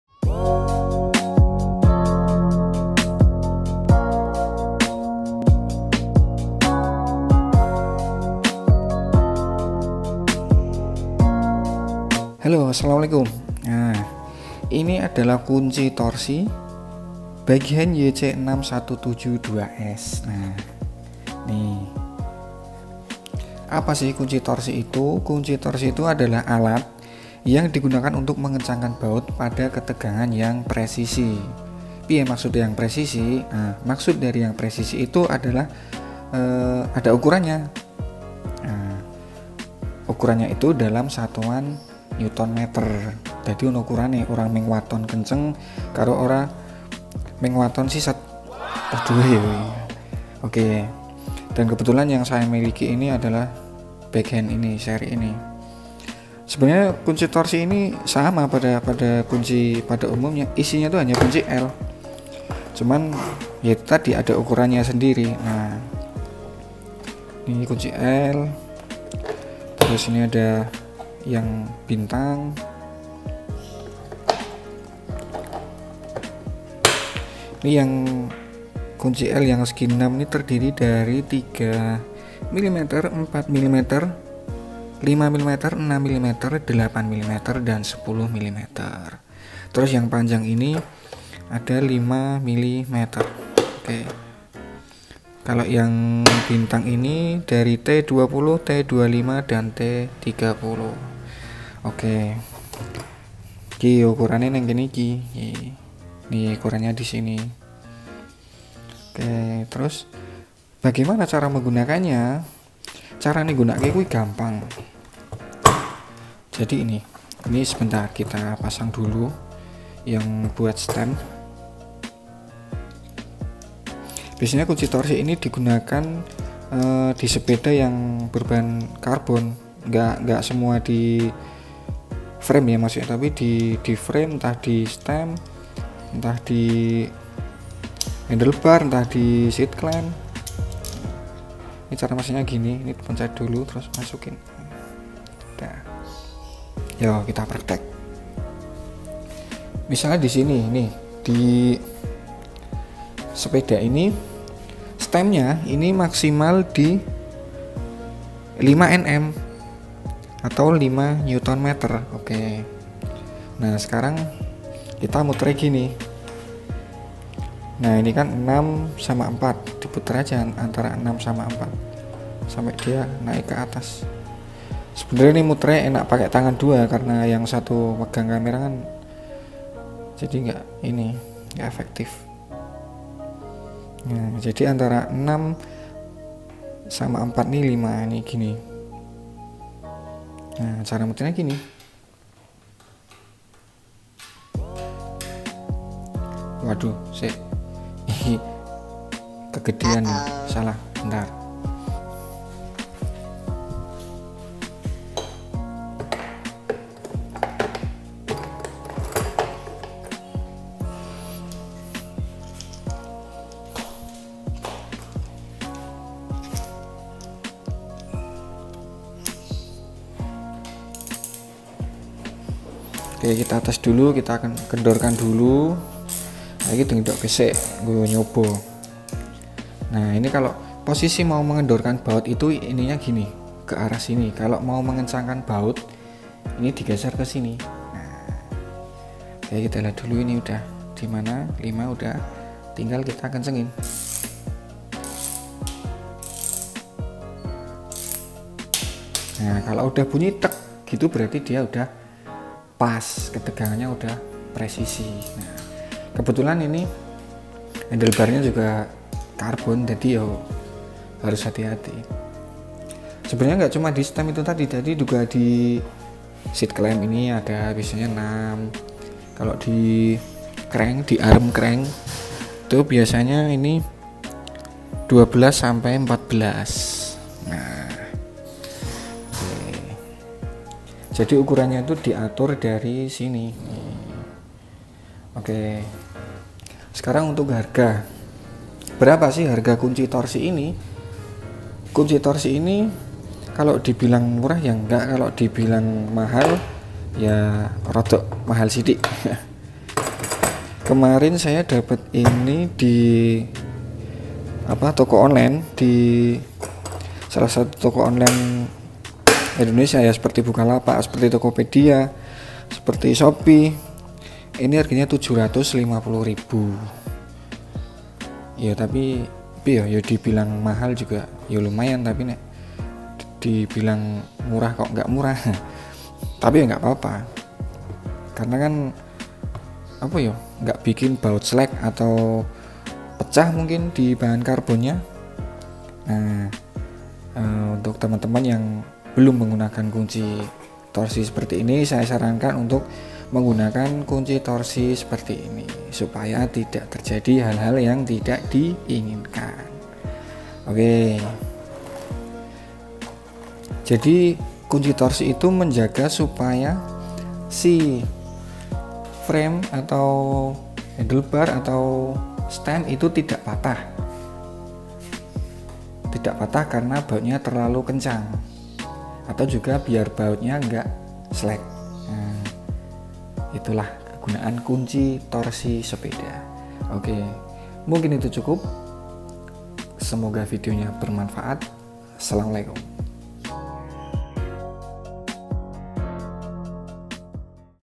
BICYCLE TORQUE WRENCH || BIKE HAND YC-617-2S || HOW TO USE || KUNCI TORSI SEPEDA
Fungsi dan cara menggunakan kunci torsi sepeda . BIKE HAND YC-617-2S.